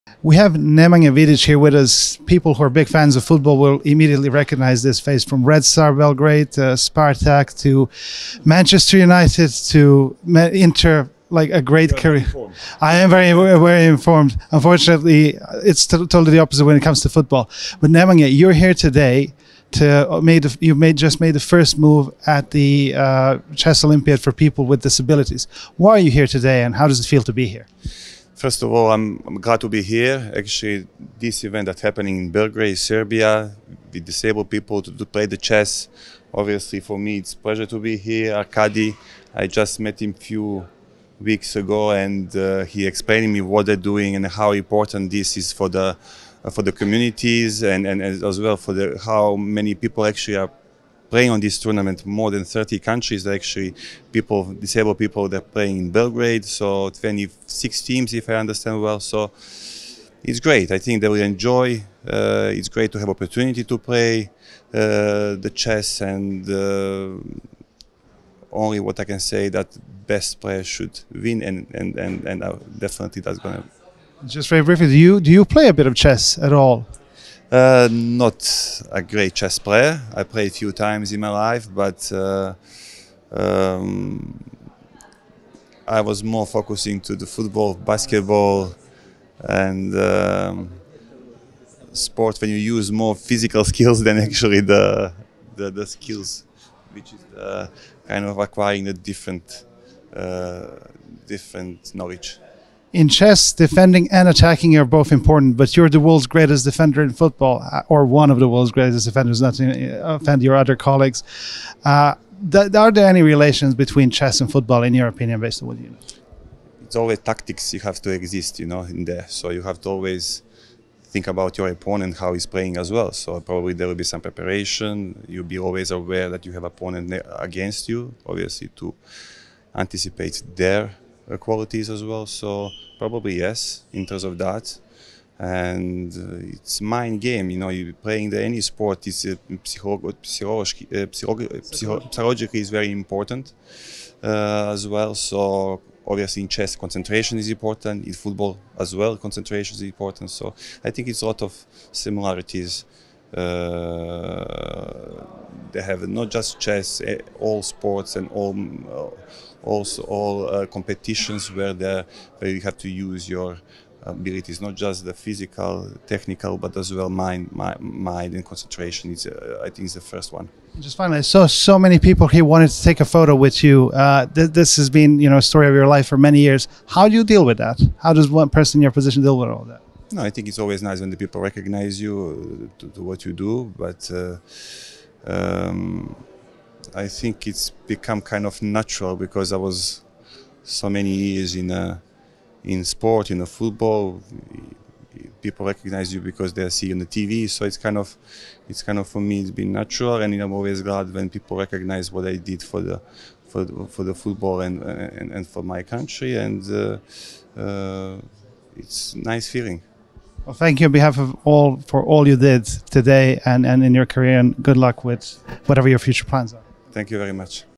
Interview with Nemanja Vidic | FIDE Chess Olympiad for People with Disabilities
«I think there are a lot of similarities between football and chess. There are tactics in both games; you have to always think about your opponent, and how he's playing, as well there will be some preparation. In chess, concentration is important, and in football as well,» said Nemanja Vidic, a Serbian former professional footballer.